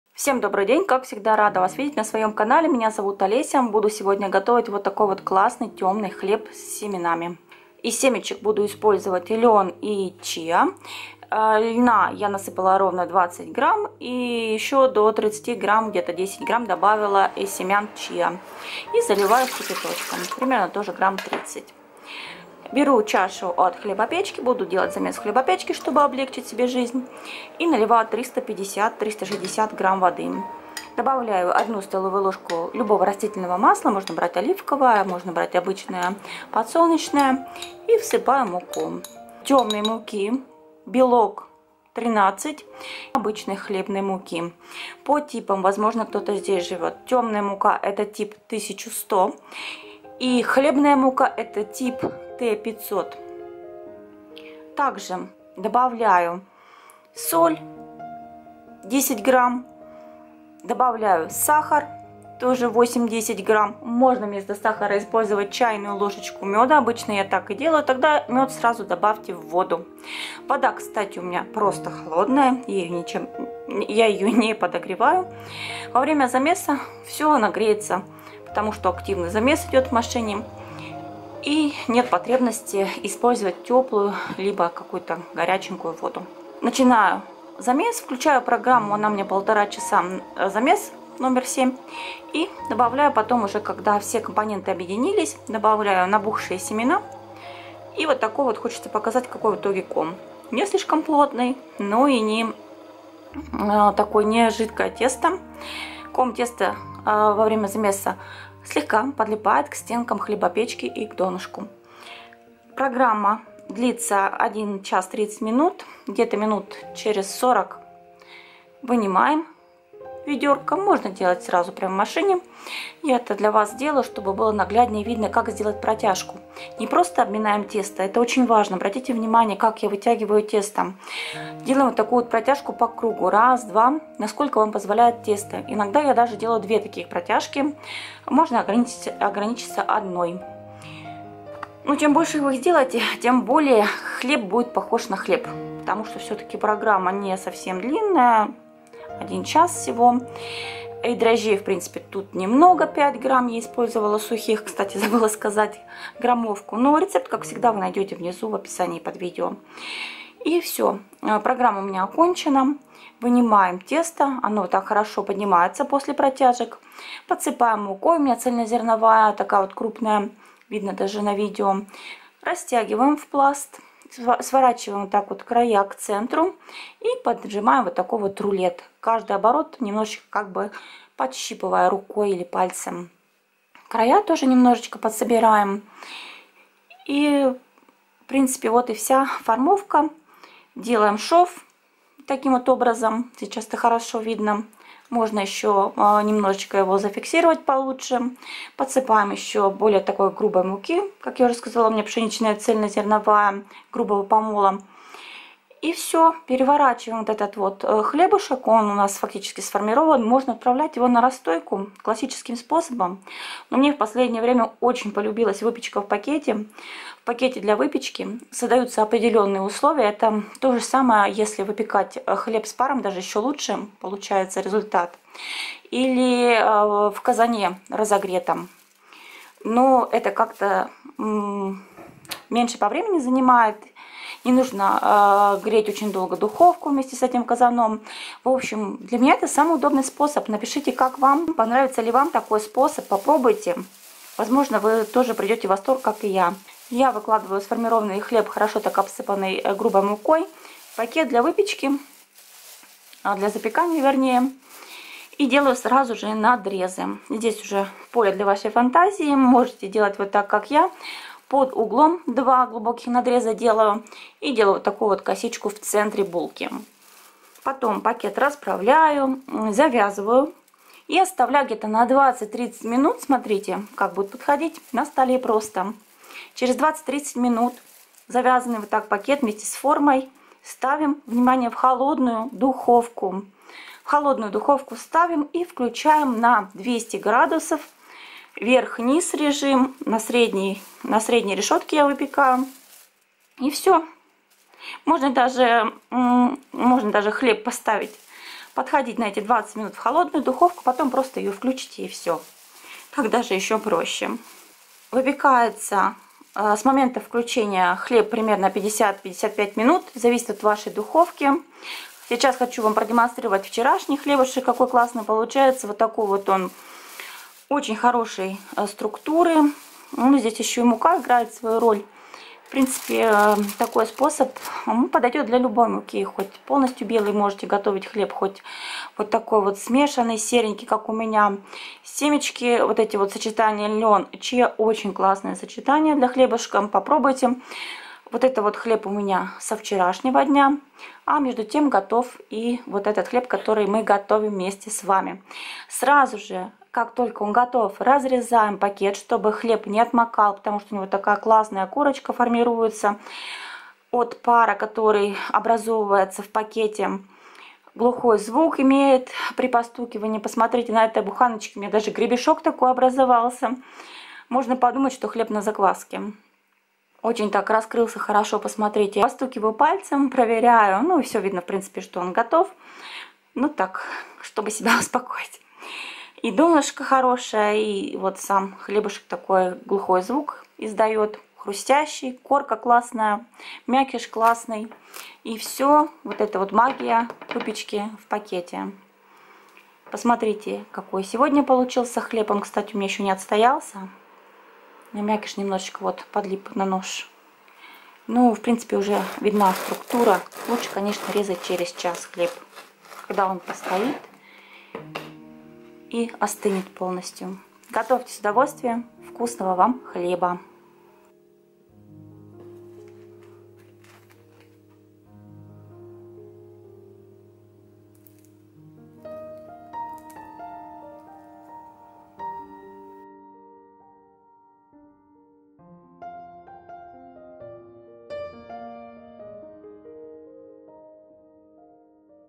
Серый хлеб с семенами, лёгкий рецепт и небольшие хитрости!
👉Как приготовить быстро и вкусно тёмный хлеб,пошаговый видео рецепт.
Если Вам понравился мой рецепт,чтобы я об этом узнала обязательно ставьте "палец вверх! и пишите об этом в комментариях!
СЕРЫЙ ХЛЕБ СО ЛЬНОМ И ЧИА
Рецепт
тесто:
350 гр мука пшеничная тёмная хлебная (белок 13, тип 1100)
250 гр мука белая пшеничная (у на тип т-500)
350-360 мл вода
8-10 гр сахар или без (на видео без) или 1 ч.л мёда
5 гр дрожжи сухие
10 гр соль
1 ст.л растительное масло
20 гр лён
10 гр чиа